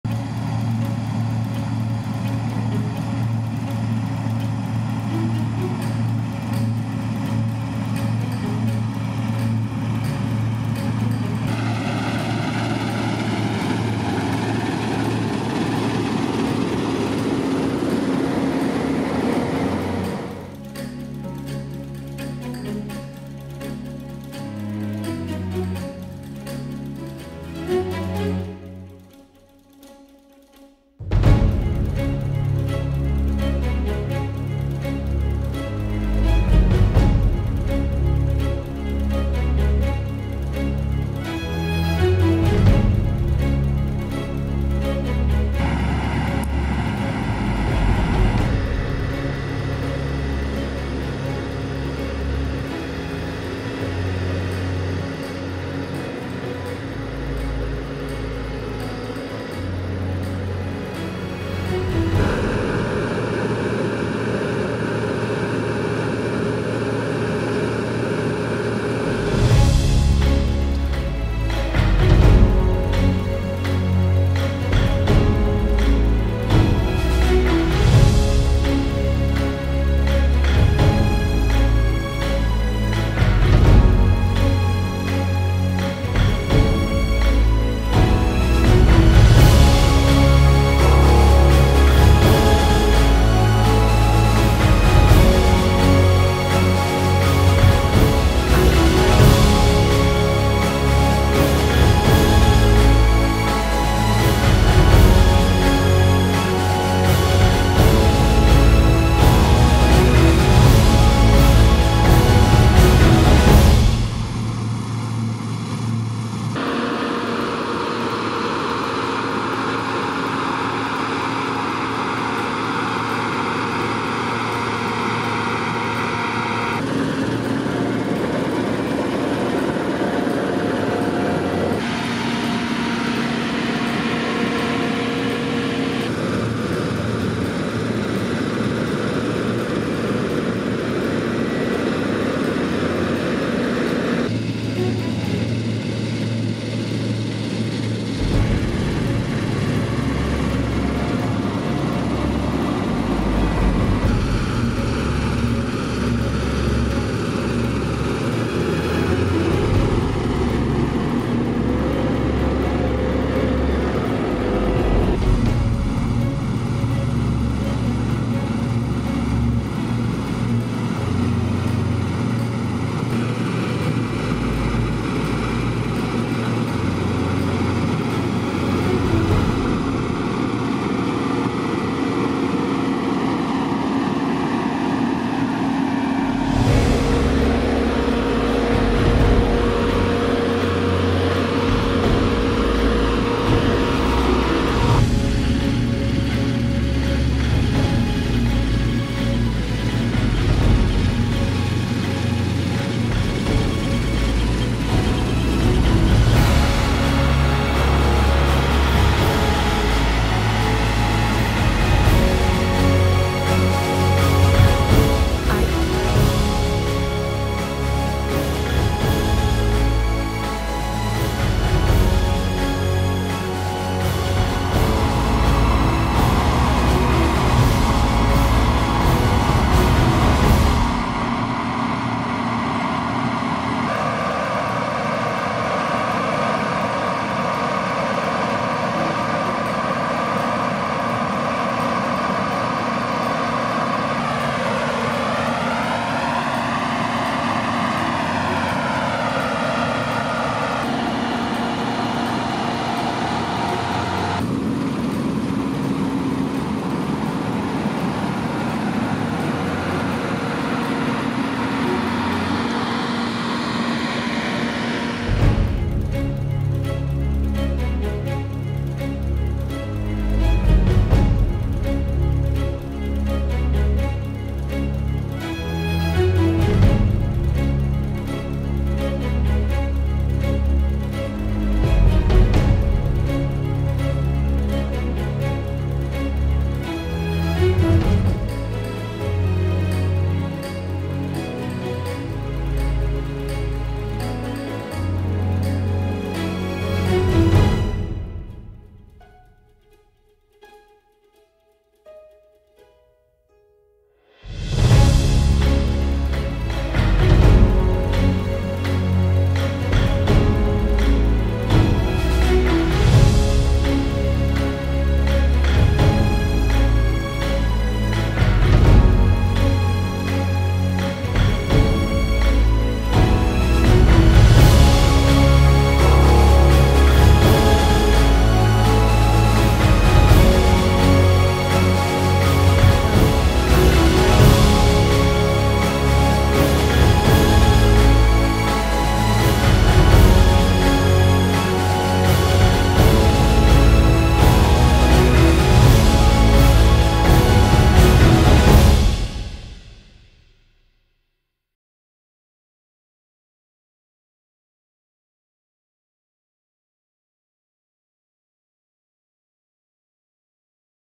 MOISSON des BLES 2023 - 100% NEW HOLLAND
Retour en image sur la moisson de blés 2023 avec un ensemble tracteur et moissonneuse batteuse 100% New Holland.